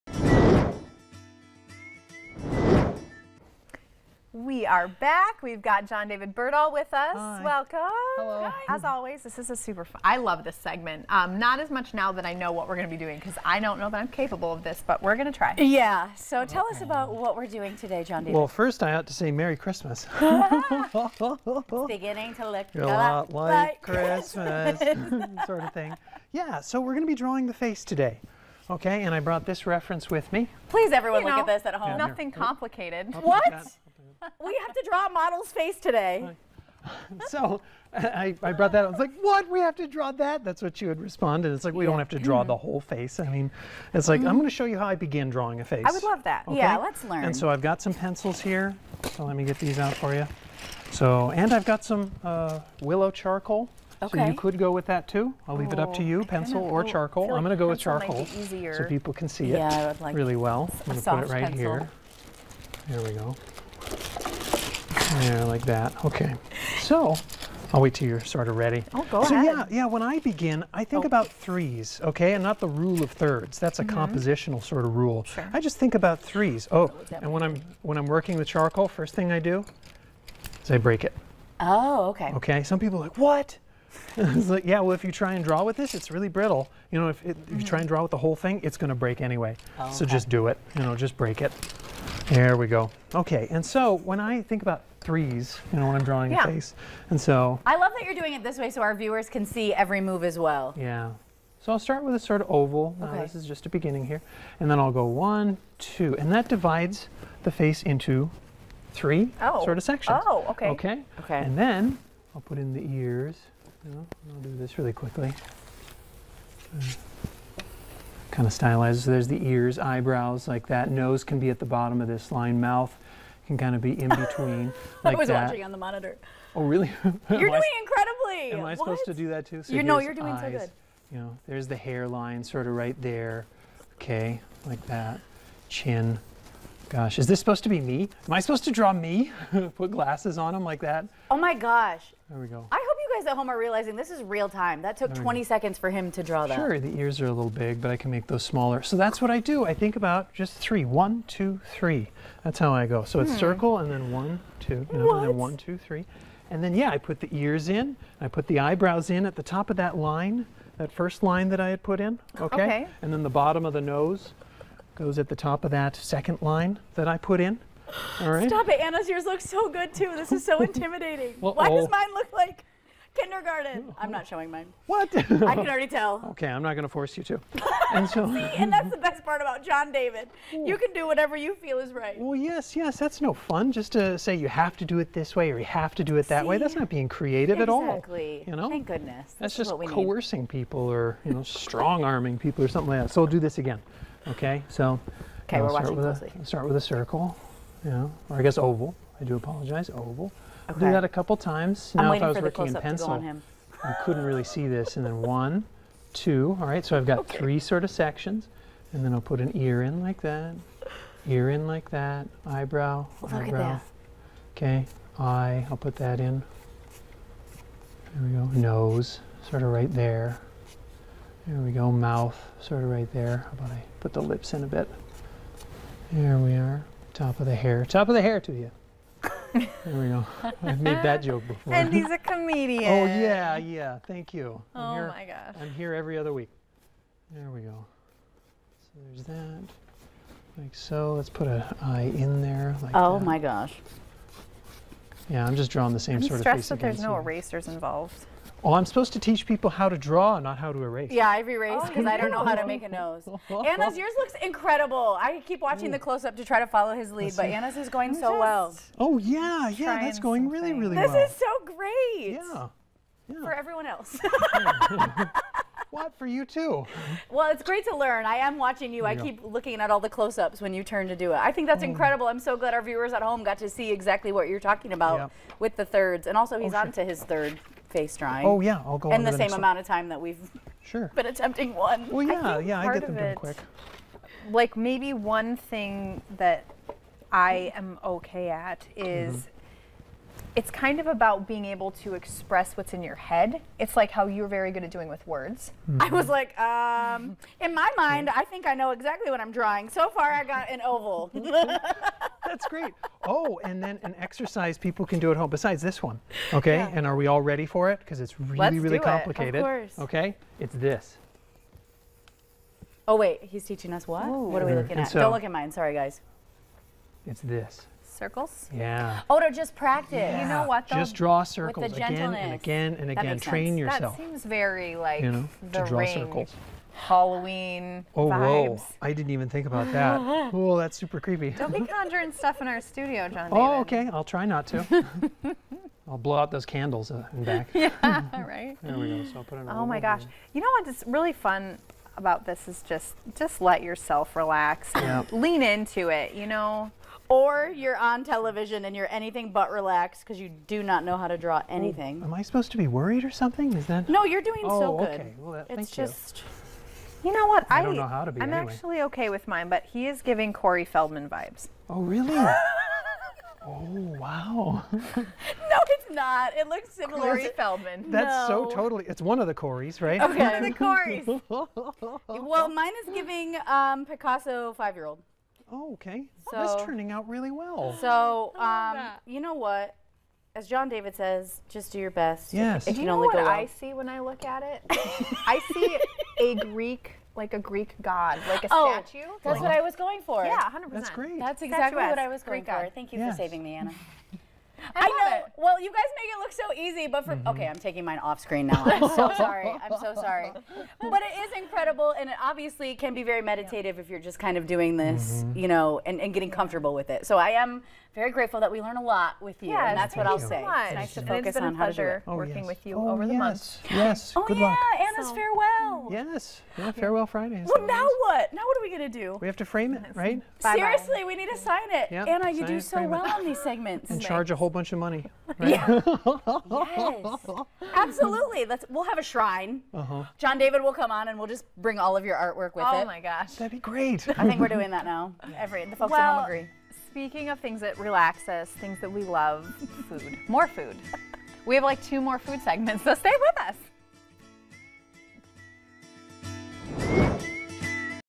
North Dakota Today - Drawing the Face
Discover the captivating world of modern portrait drawing, where mood elements, intellectual tension, and personal engagement come together to create stunning artworks! This art form goes beyond mere technique, allowing the artist's personal craftsmanship to shine through!

With each stroke of the pencil, the artist captures the essence of their subject, creating a timeless piece that reflects their unique perspective! Dive into the realm of modern portrait drawing and experience the power of self-expression through this captivating art form!